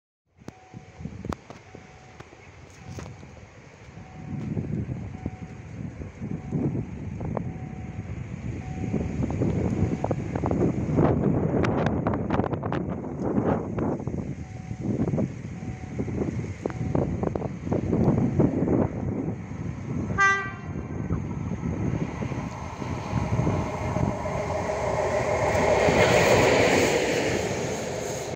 15D-114 Rp1
Luzak Silva za chwilę wjedzie do Wilkołaza od strony Kraśnika, przed południem prowadził skład kleju do Mielca.